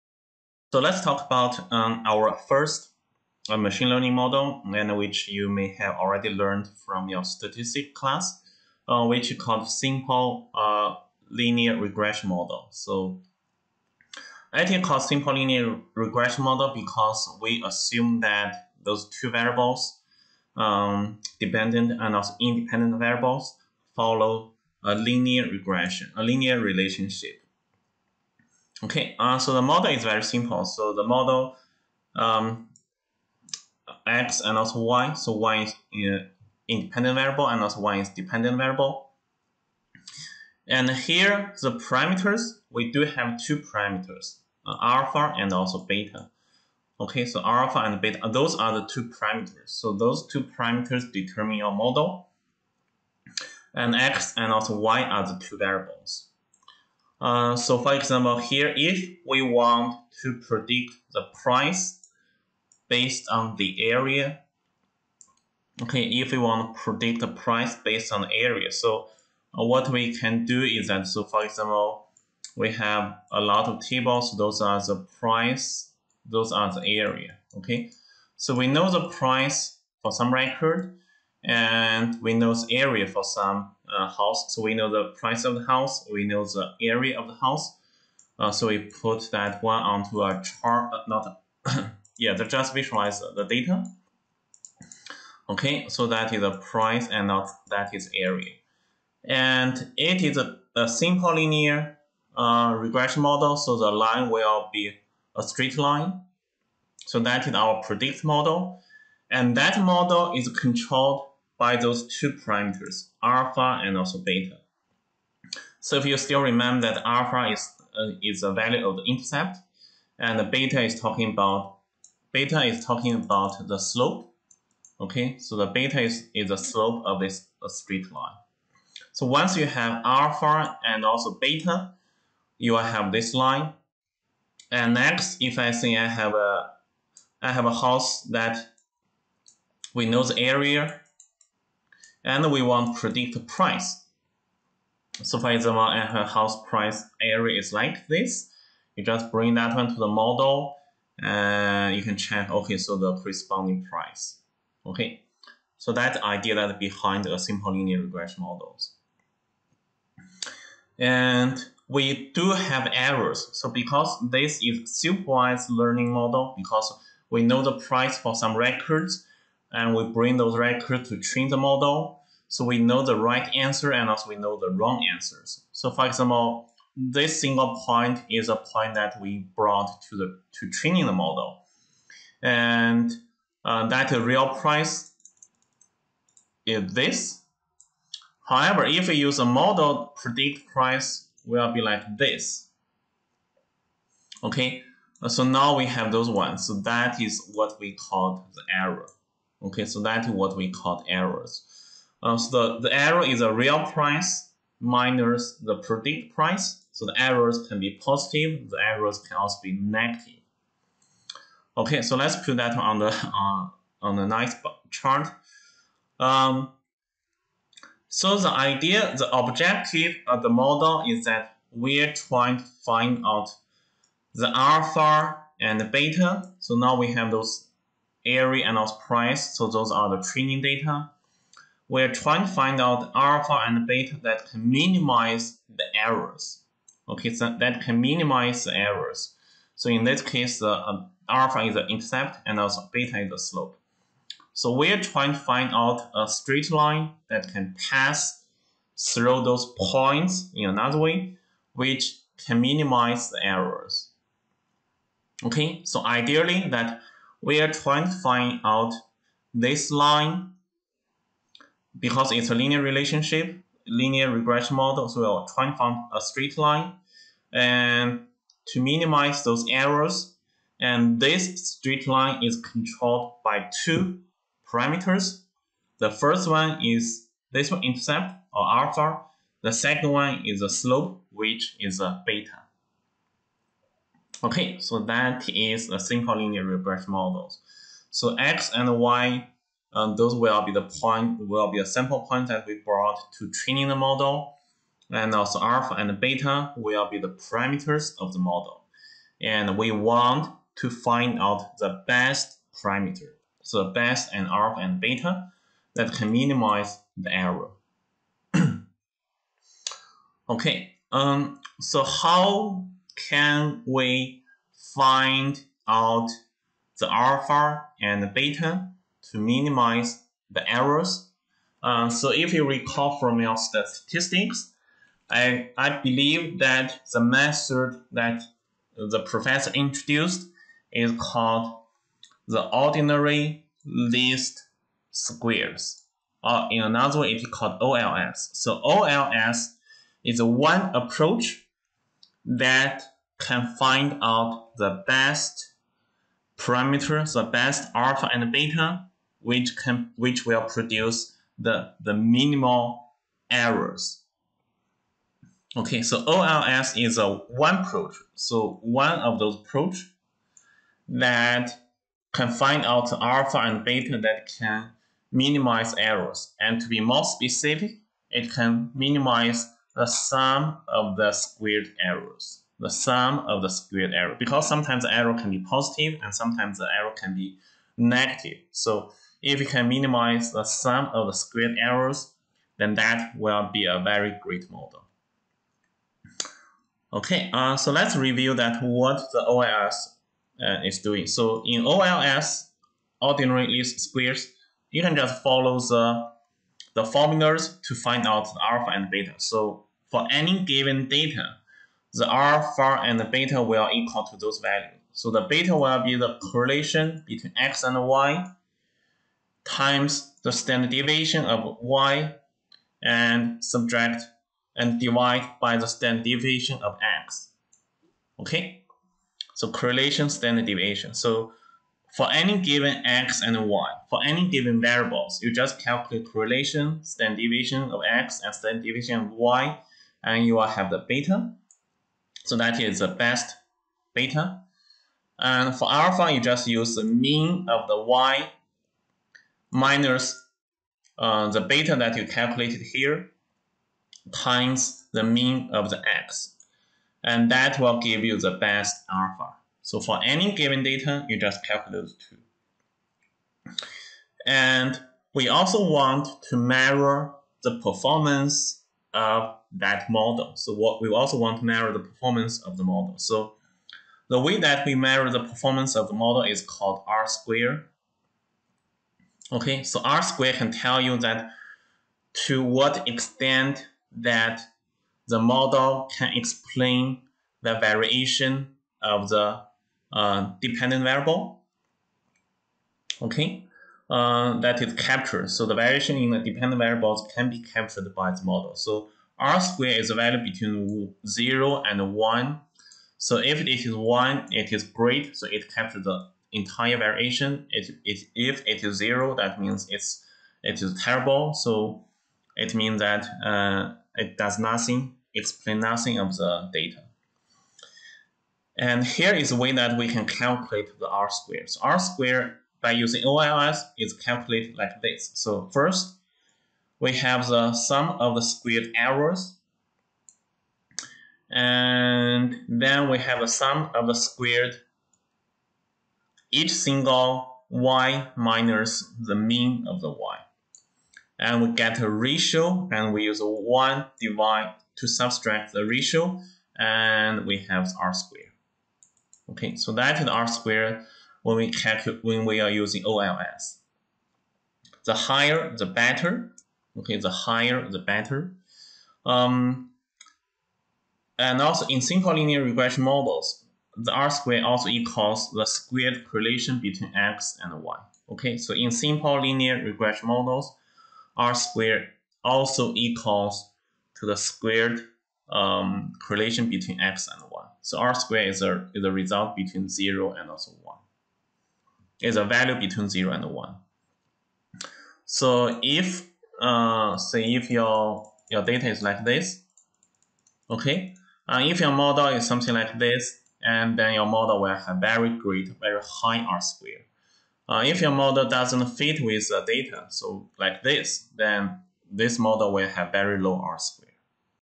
Lec 3.3 Simple Linear Regression and OLS
Table of Contents:

00:00 - Simple Linear Regression
00:28 - The Model
05:11 - The Ordinary Least Squares (OLS)